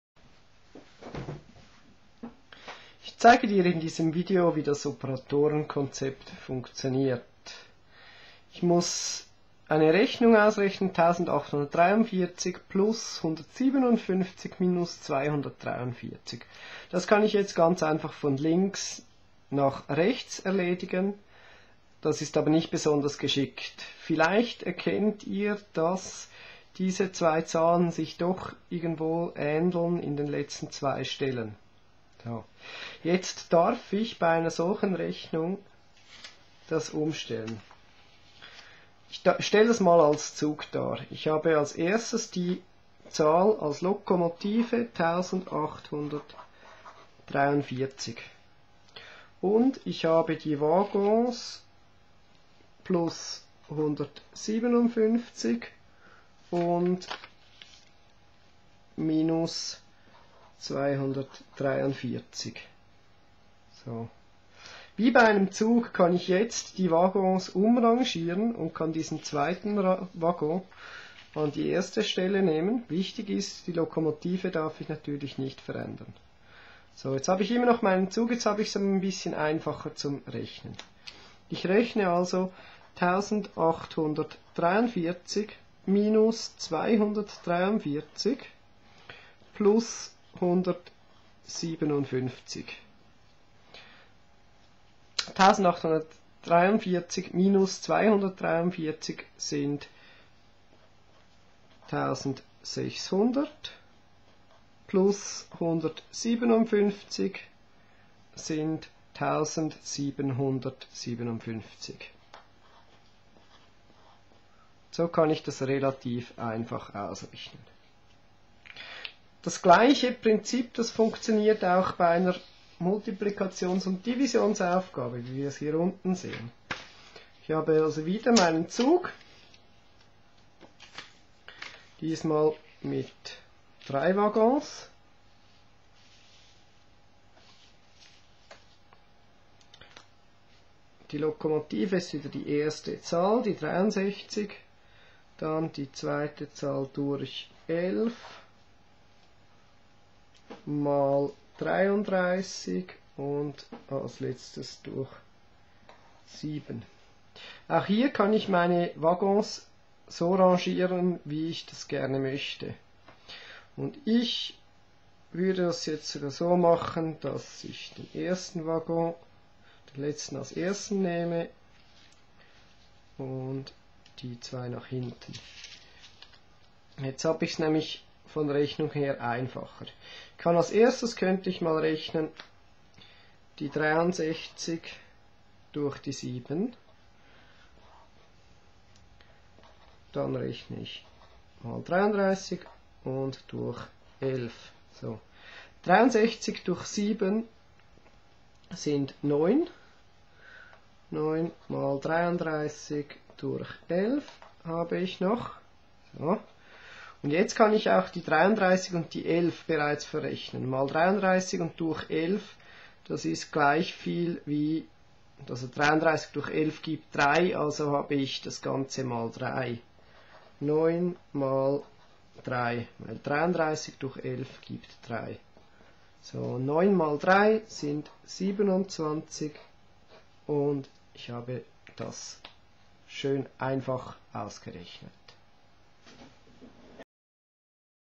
Operatorenkonzept
Zwei Beispiele zum Operatorenkonzept wie es bei Aufgabe 6 S.50 und Aufgabe 8 S.62 angewendet wird.